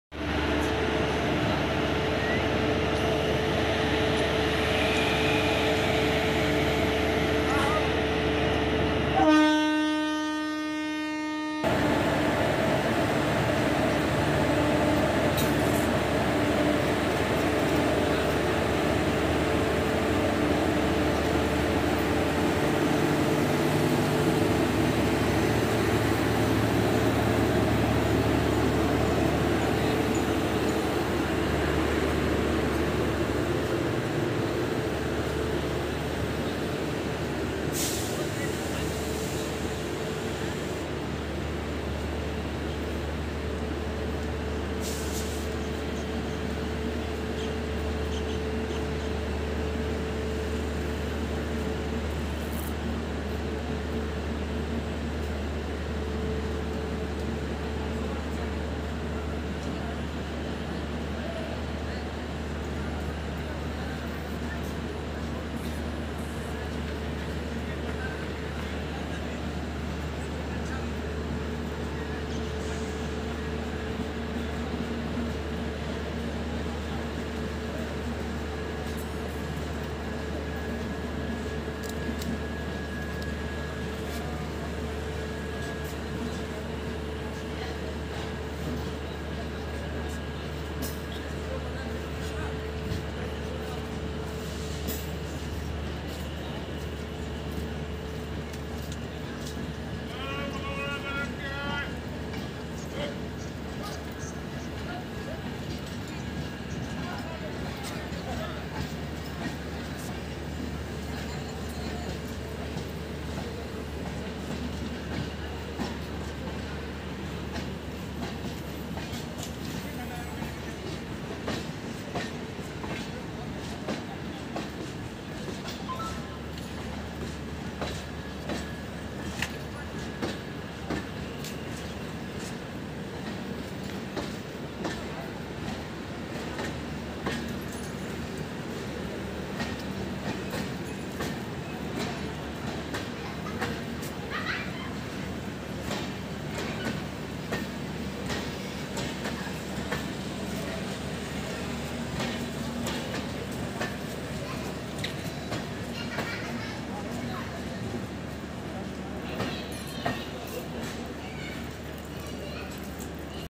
22103 अयोध्या कैंट सुपरफास्ट (लोकमान्य तिलक टर्मिनल - अयोध्या कैंट) Diverted Train Via Pratapgarh jn
प्रतापगढ़ जं के रास्ते निकली जौनपुर, प्रयागराज के रास्ते मुंबई जाने वाली ट्रेन। 22103 अयोध्या कैंट सुपरफास्ट (लोकमान्य तिलक टर्मिनल - अयोध्या कैंट) Diverted Train